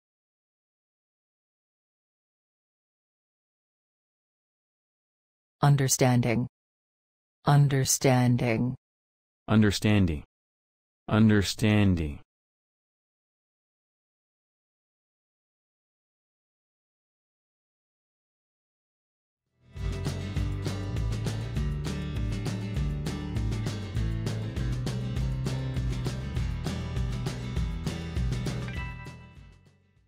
كيف أقول فهم باللّغة الإنجليزيّة
كيف أقول بشكل صحيح فهم باللّغة الإنجليزيّة؟
ما هي التّرجمة الصّحيحة لكلمة فهم من العربيّة إلى الإنجليزيّة؟
كيف تكتب بشكل صحيح كلمة understanding?

شكرا جزيلا!